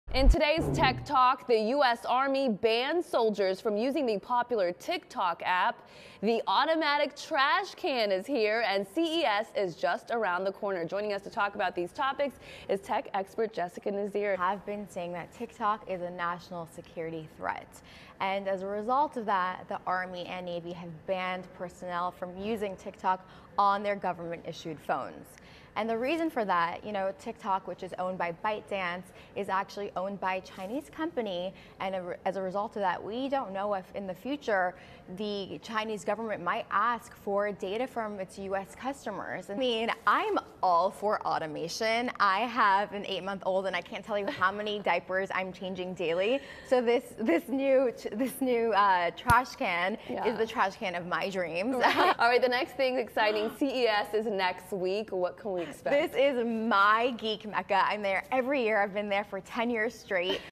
Tech headlines of the week
The US Army bans soldiers from using TikTok over security worries; CES, the biggest tech event of the year is right around the corner, and I’ll give you a glimpse at the trends; and Townew automatic trash can is here.